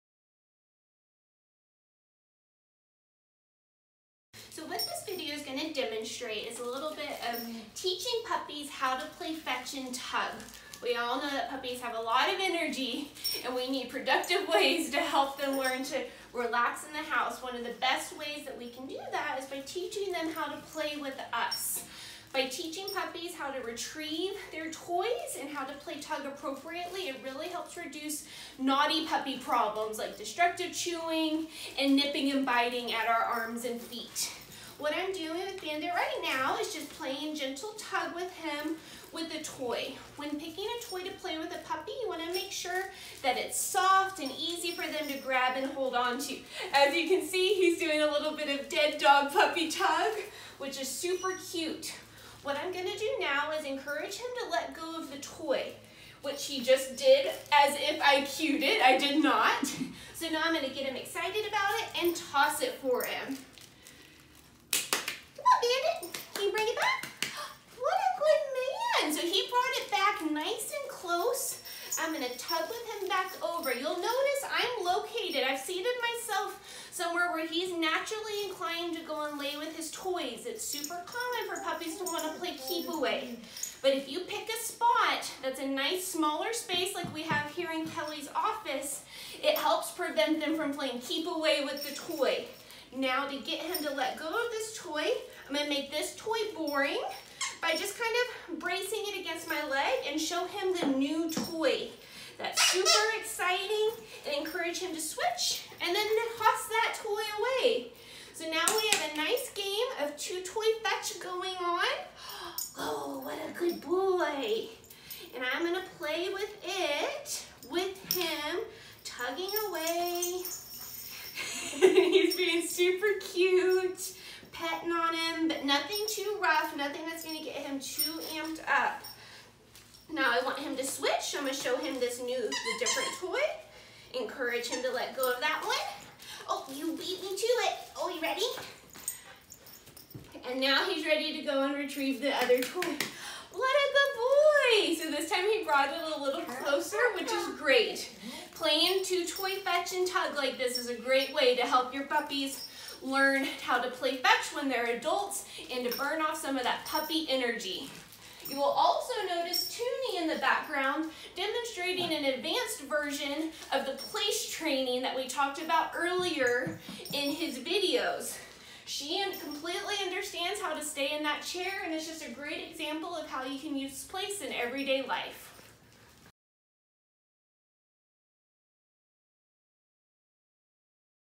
Teaching Your Puppy to Play
Learn how to teach your puppy to play properly to keep unwanted puppy behaviors away!